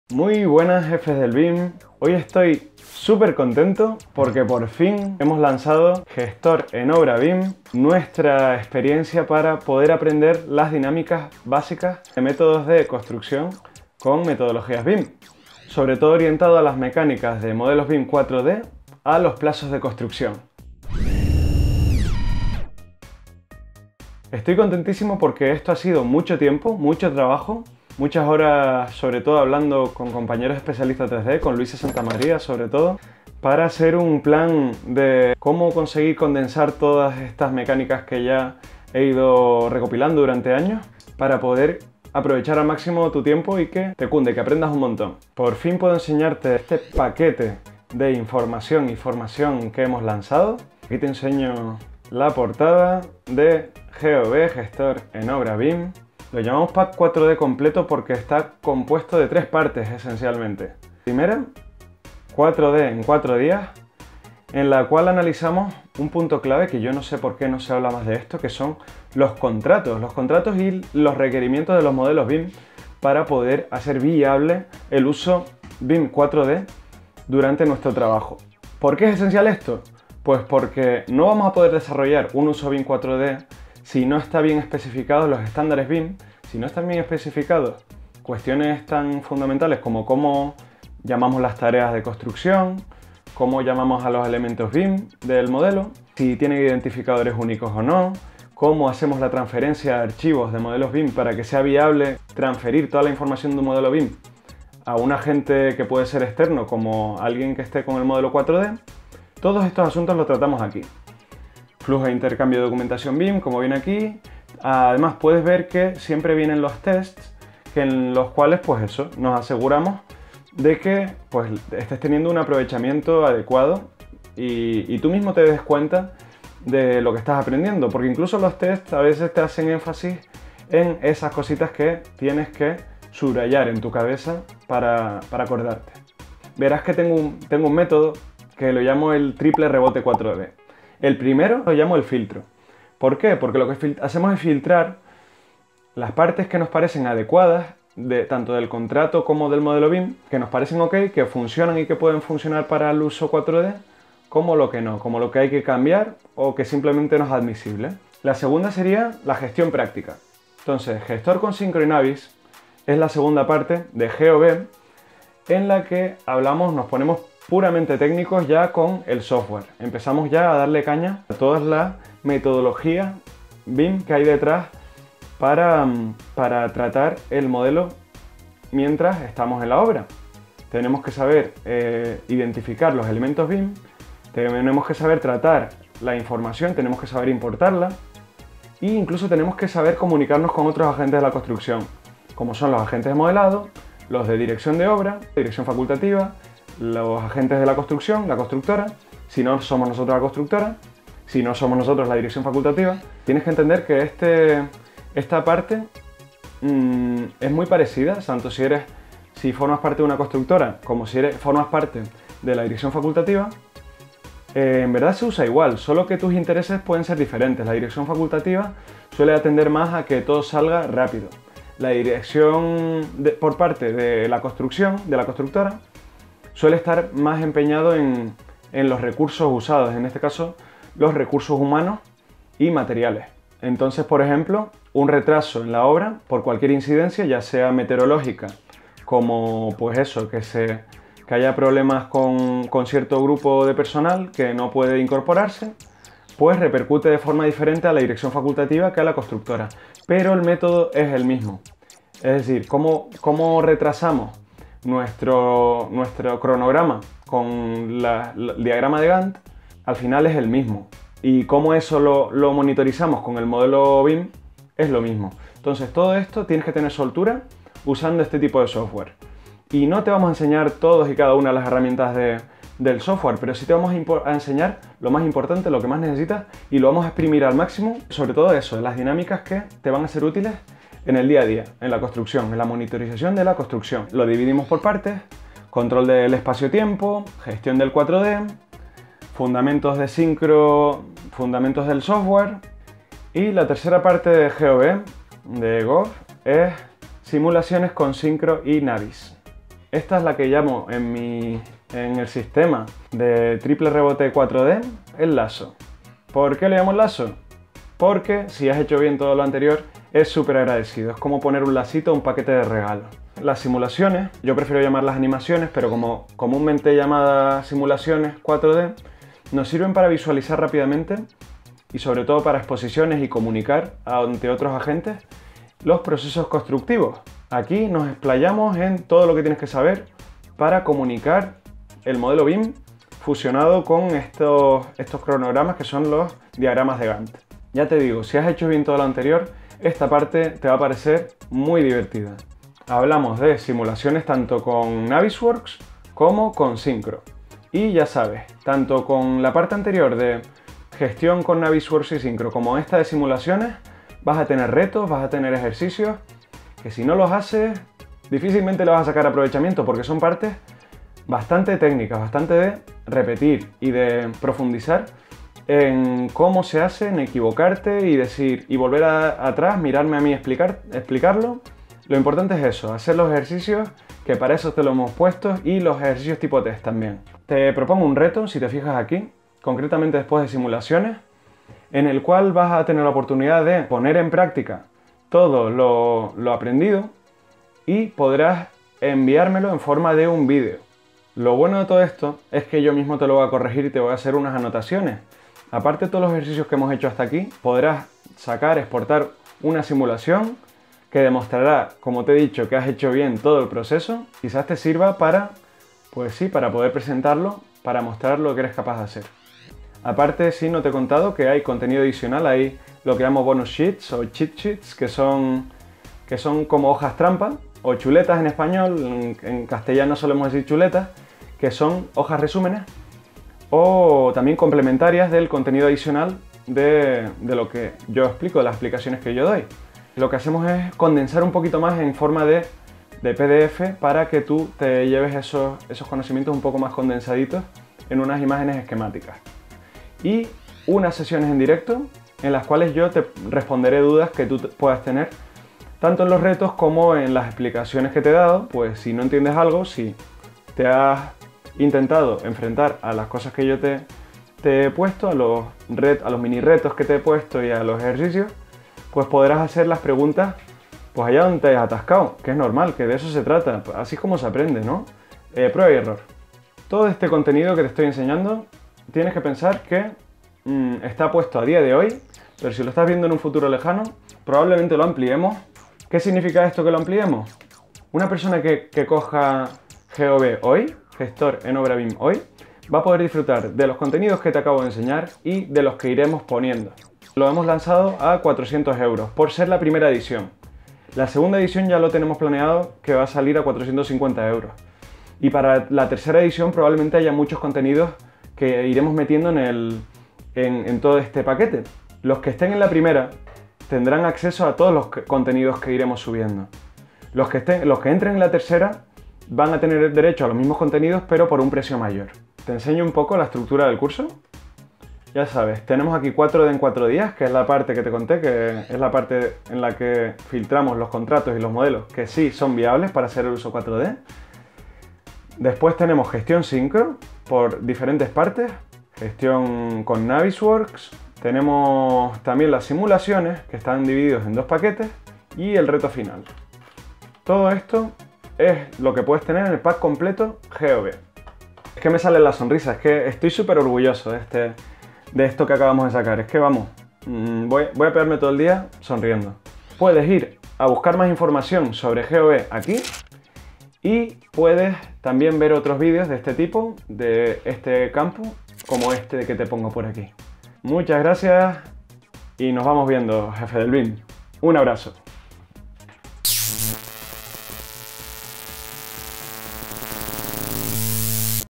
Abrimos GOB. Échale un vistazo🔓
Gestión de Obra con BIM es el proceso con el que vas desde "¿qué hacer con este modelo que no es mío?" a tener una planificación completa del mismo con un modelo 4D. La idea es que sea tu guía y tu sitio de referencia de consulta desde el momento del proyecto de ejecución hasta la obra cuando estés trabajando con metodologías BIM.

Lo hemos dividido en 3 bloques:

4D en 4 días: que se ocupa de ese momento en el que te enfrentas a un modelo, mejor o peor hecho, y a evaluar si es viable trabajar con él la planificación del proyecto. El objetivo de esta parte es evitarte perder dinero y/o tiempo en contratos BIM.
Gestión con Synchro y Navisworks: donde trabajamos con dos de los software más solicitados en el mercado en cuanto a BIM 4D. Podíamos haber seleccionado uno u otro pero en el mundo real a veces el cliente elige por ti, este bloque te servirá para estar preparado con cualquiera de los dos software.
Simulaciones: aquí ves las herramientas que te permiten visualizar la planificación haciendo una animación de la construcción. También gracias a este bloque tu cliente podrá entender las diferentes opciones.